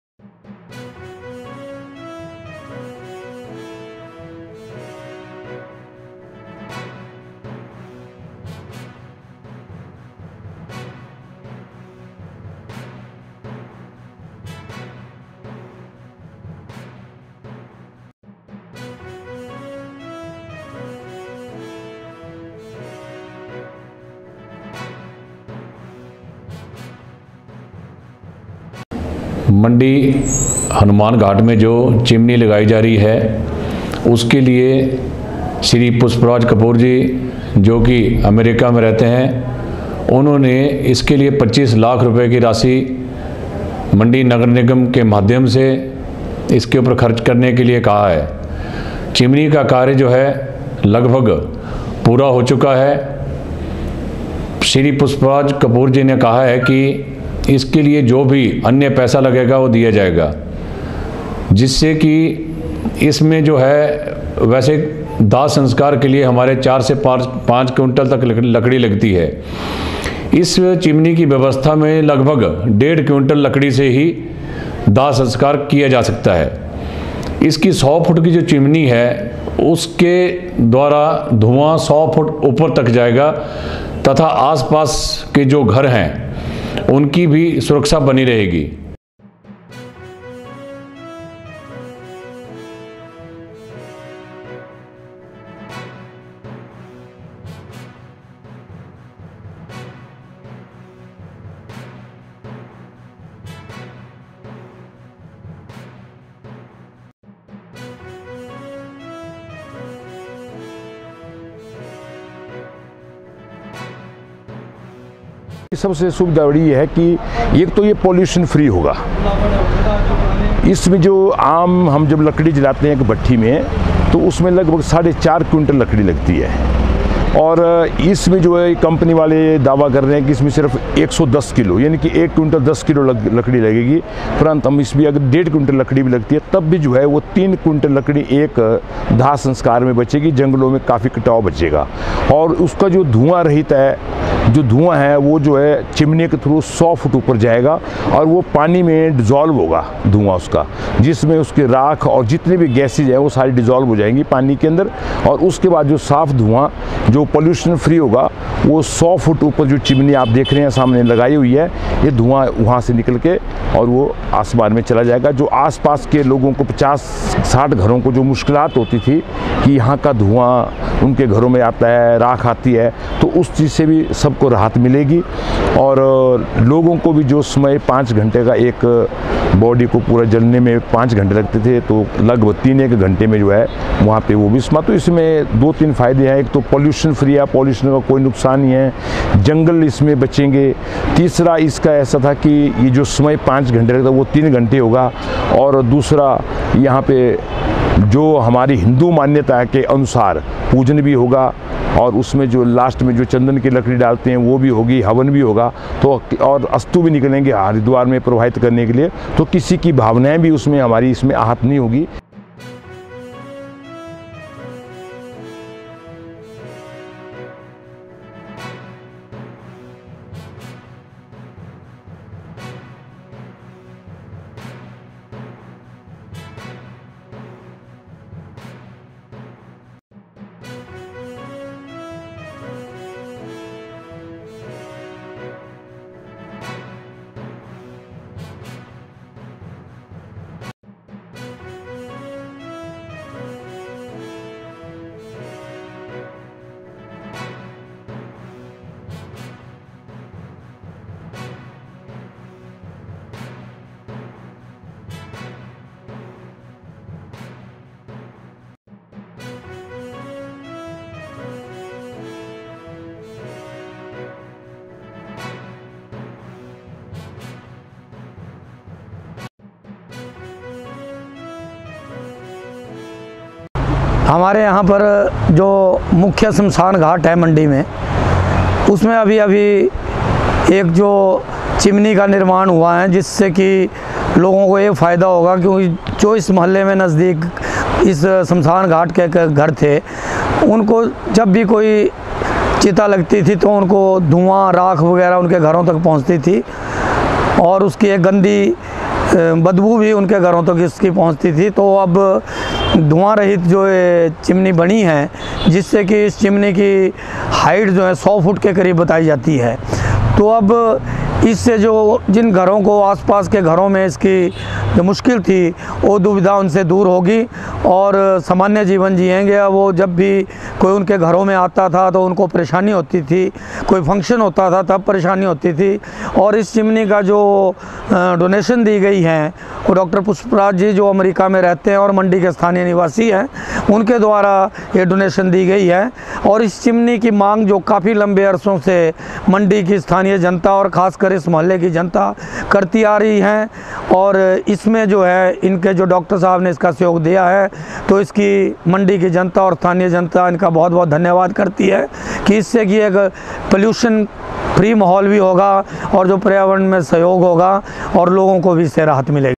Green crematorium || Mokshadham || Mandi
Green crematorium is being constructed at Mokshadham on Hanuman Ghat in Chhoti Kashi Mandi. This Green Crematorium is being constructed by Municipal Corporation Mandi with the help of social worker Dr. Pushp Kapoor, a resident of Mandi city in America and social organizations. The construction of the Green Crematorium was started 20 days ago. Now it is almost ready. Green crematoriums will use less than half of the wood normally used for cremation. At the same time, this will also strengthen the efforts for environmental protection. In this, a 100 feet high chimney has been installed in it for smoke emission.